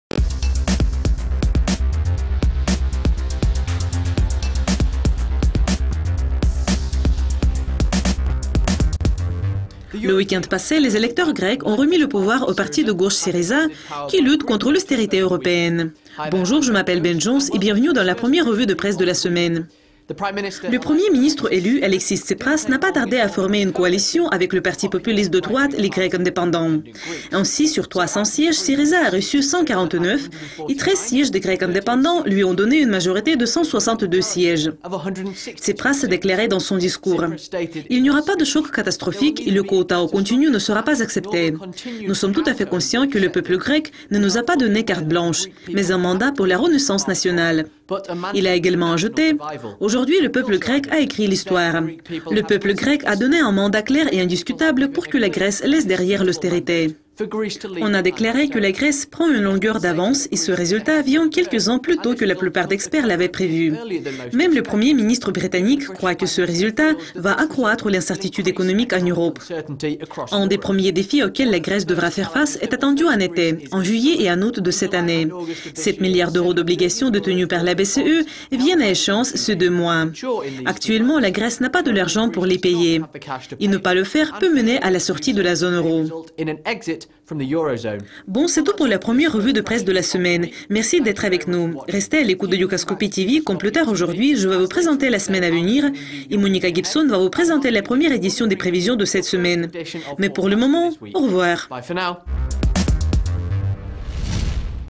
Grèce contre l'austérité - 01.02.2015 - Dukascopy Press Review
Une coalition a été formée comme Syriza gagne la majorité des sièges.